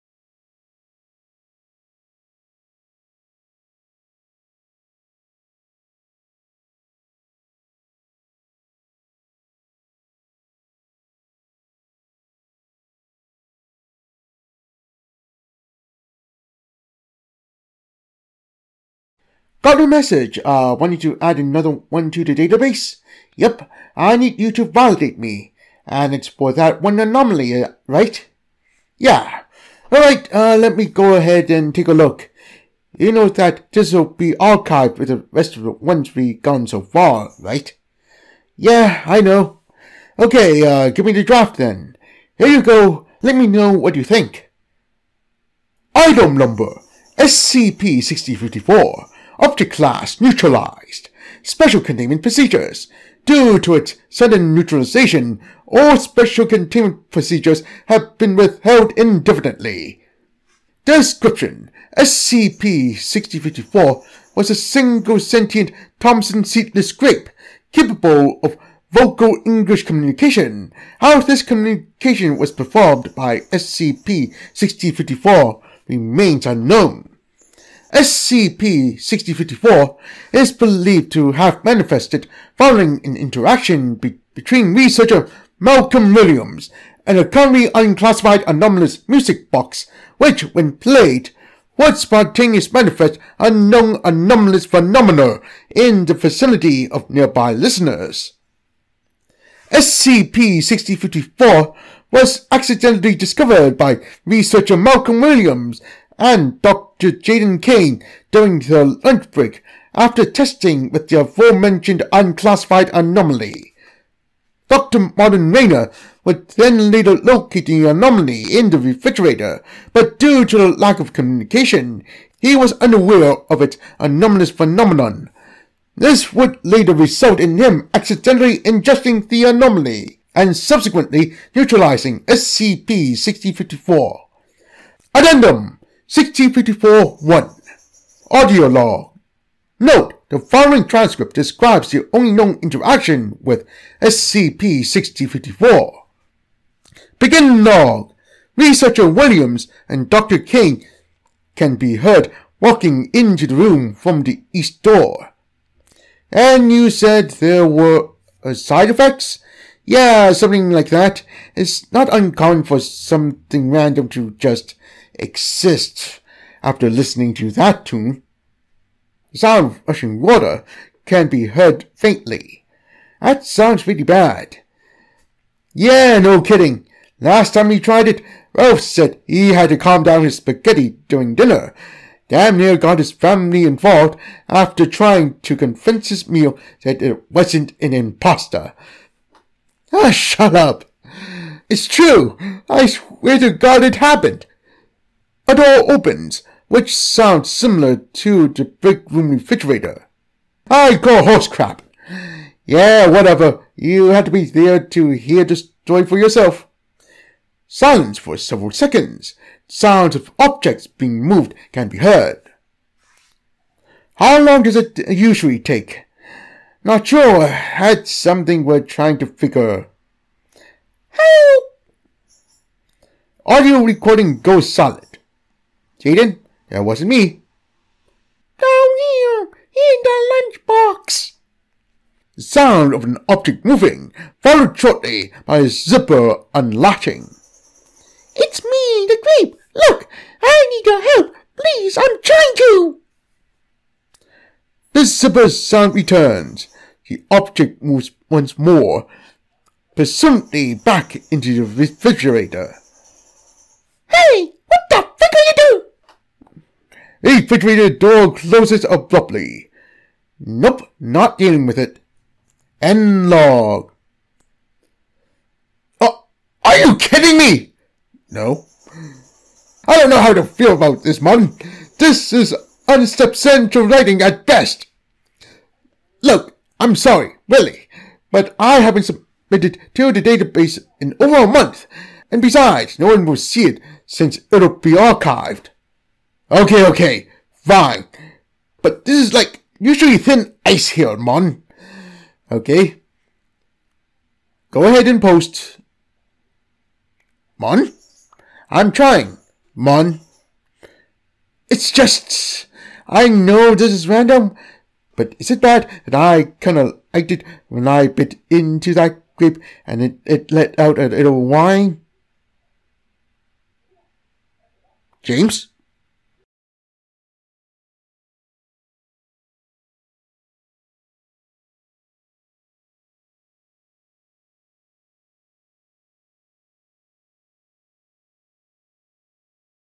SCP 6054 Homophone Gramophone
SCP-6054 was a talking grape that seem to come into being thanks to another anomaly, the titular gramophone.